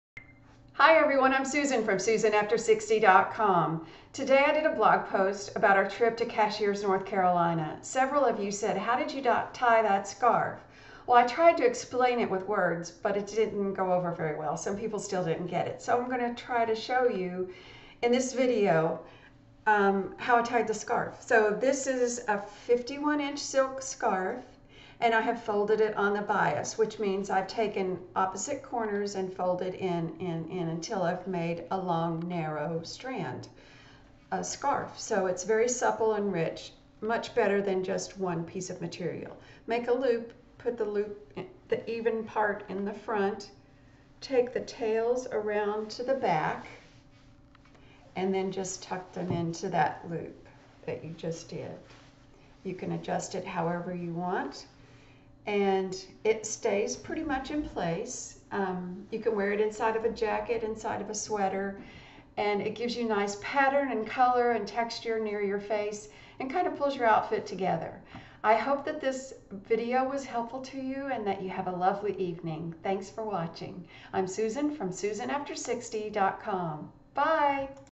An Easy Way to Wear A Scarf
The blog post showing this scarf worn as I'm showing you in this video is
The following blog post shows you how to fold a large silk scarf on the bias...